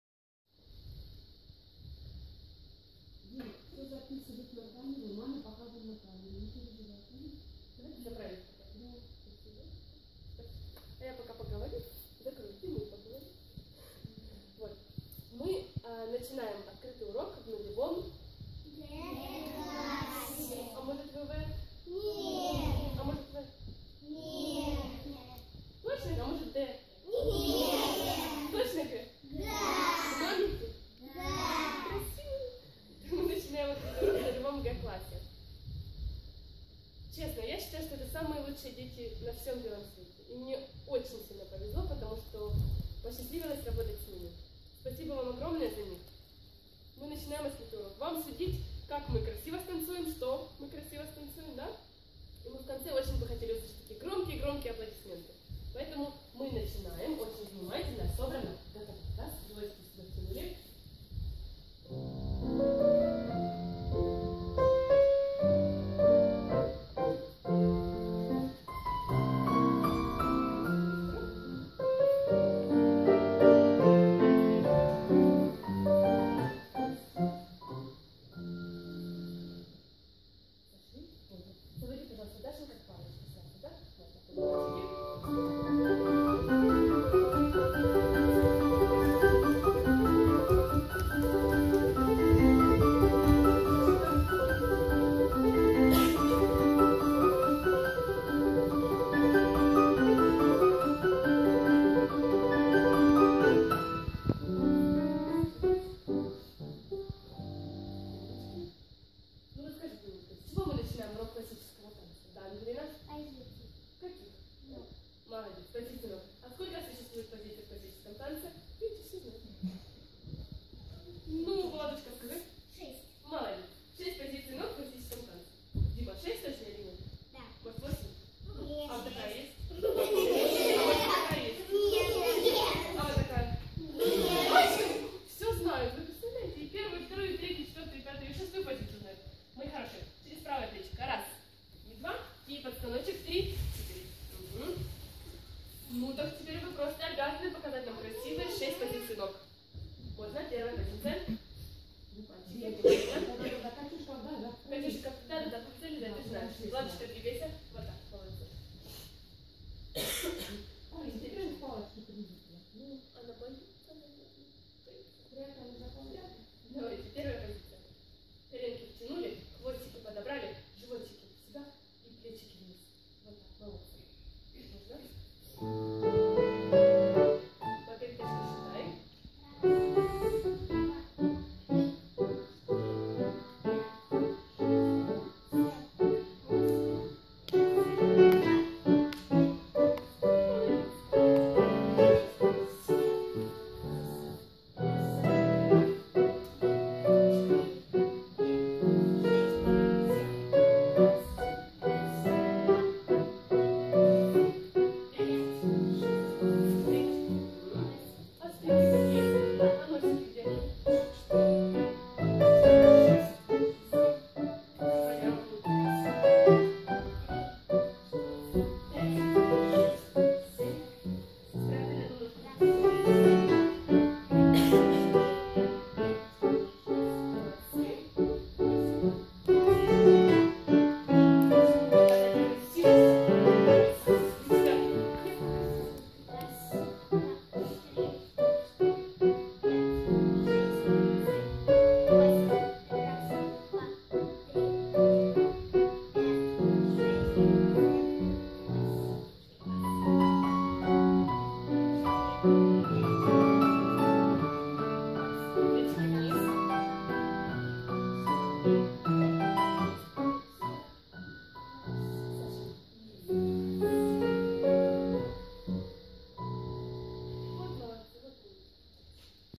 Pre-Ballet 1 Class (Age 6) Classical Dance Lesson (part 1)
Teacher: Ievgeniia Iashchuk
Place: Kharkiv Ballet Academia

Classical Dance Lesson for 6 years old children. Organised in the way to educate them not only as ballet dancers, but what's more important, ballet dancers that live on the scene.